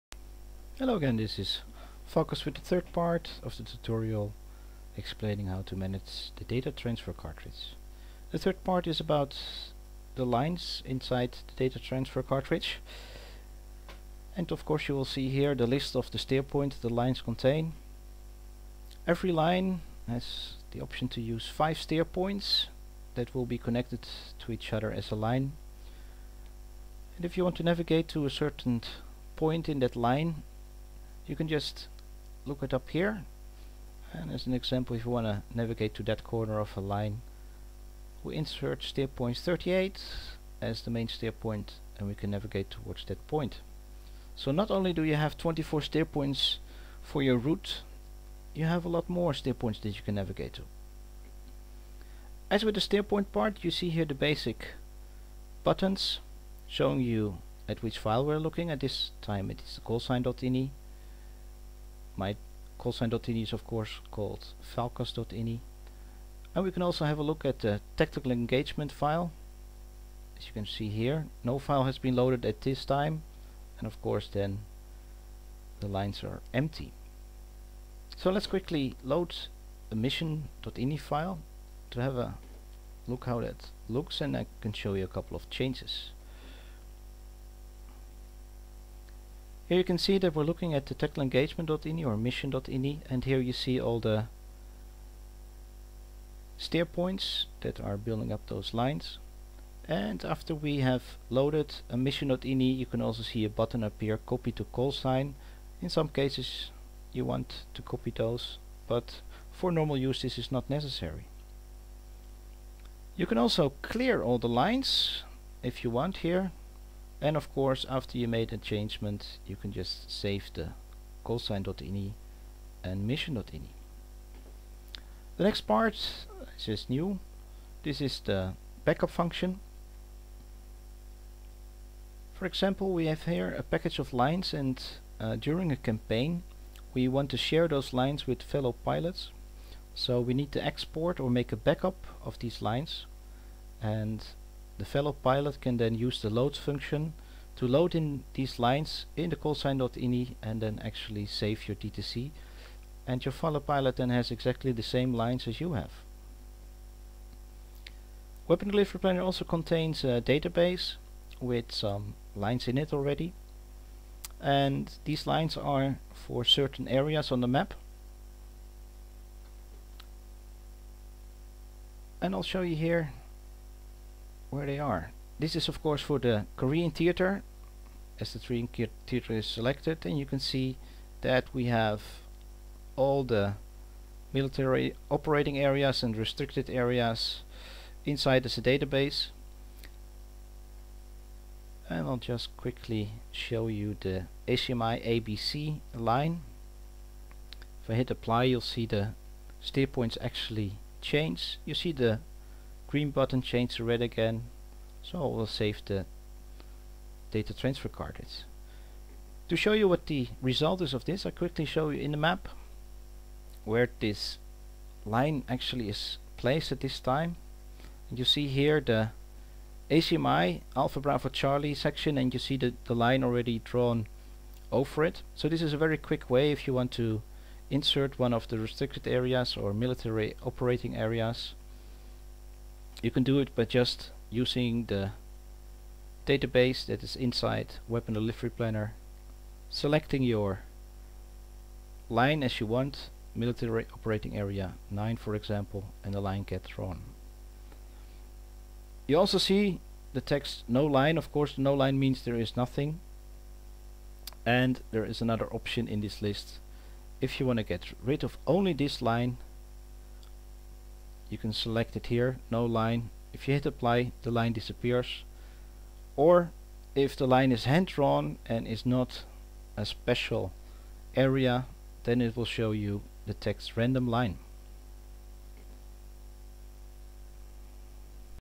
DTC management in WDP Tutorial Part3
A 10 part tutorial how to manage your DTC using WDP.
Part3 Line tab.